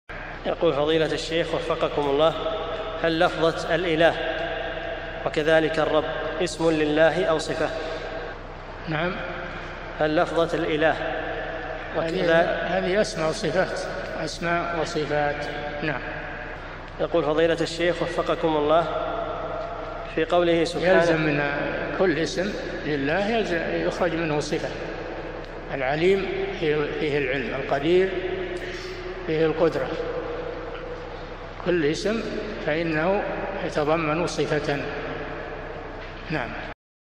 [972 -1024] هل لفظة "الإله" وكذلك "الرب" اسم لله أو صفة؟ - الشيخ صالح الفوزان
-"[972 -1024] السؤال: هل لفظة ""الإله"" وكذلك ""الرب"" اسم لله أو صفة ؟
الجواب: هذه أسماء وصفات، يلزم من كل اسم لله أن  يخرج منه صفة، العليم فيه العلم، القدير فيه القدرة، كل اسم يلزم فإنه يتضمن صفة.
للاشتراك في قناة الشيخ صالح الفوزان
سؤال وجواب من شرح الفتوى الحموية ( 1024 ) مقطع

للاستفسار والإقتراحات والدعم
رئيس الفريق
لمشروع كبار العلماء